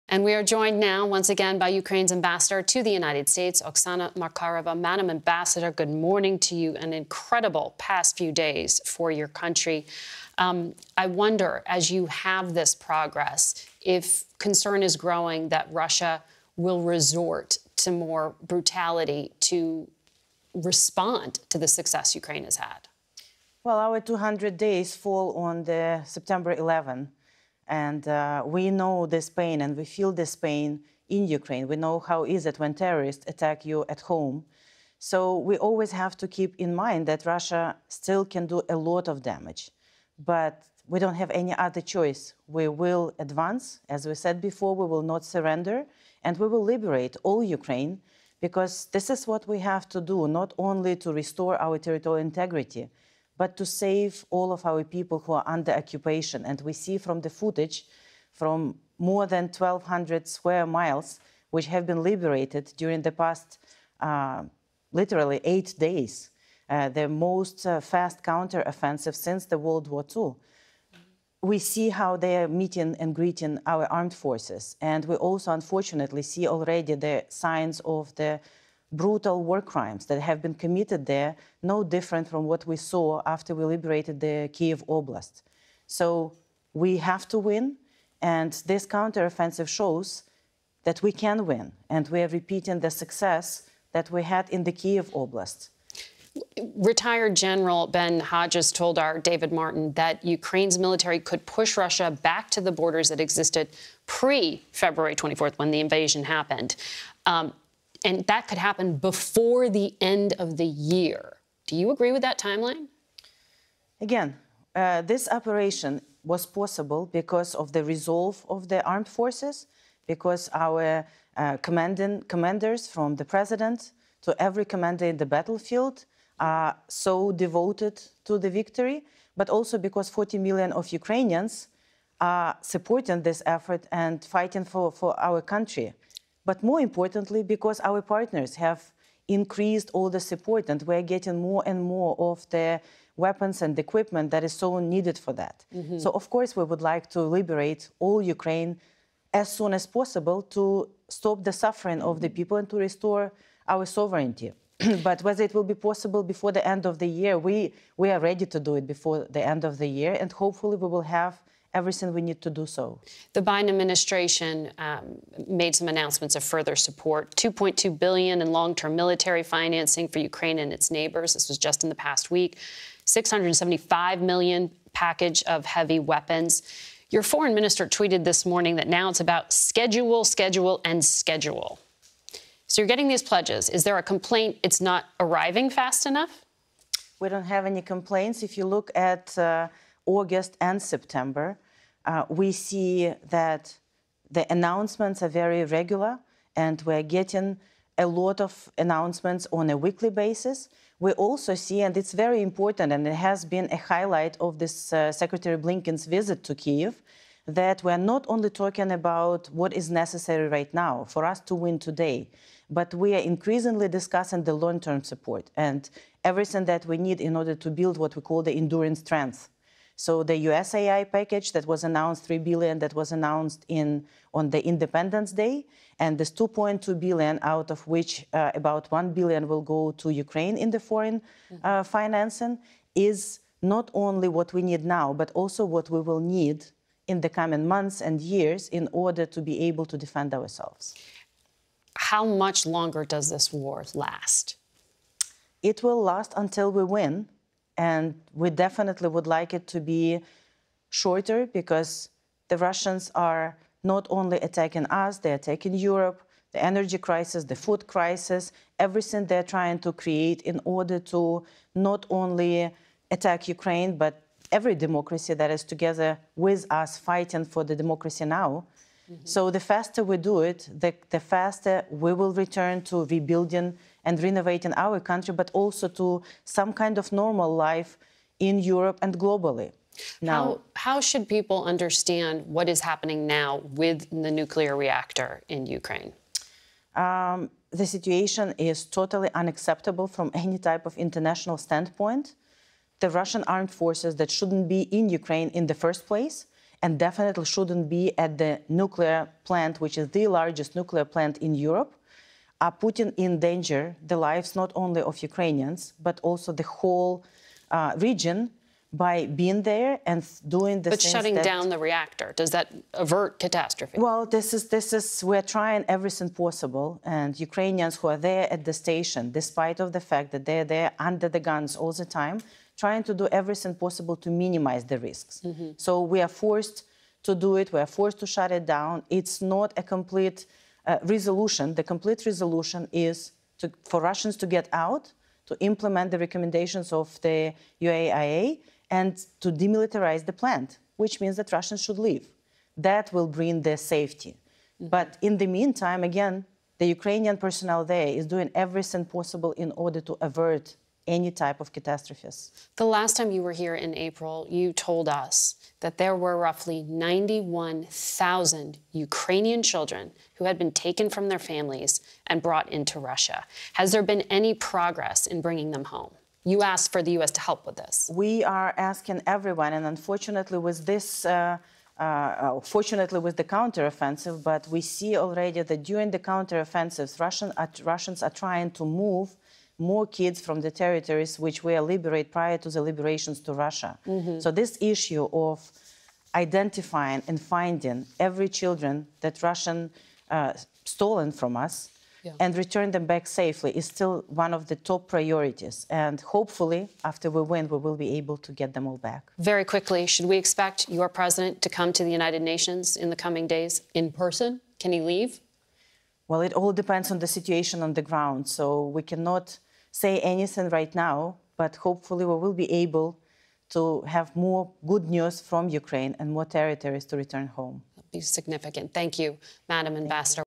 Ukrainian ambassador vows forces will "liberate all Ukraine" amid counteroffensive
Ukrainian Ambassador to the U.S. Oksana Markarova says the ongoing Ukrainian counteroffensive shows that "we can win" the war against Russia.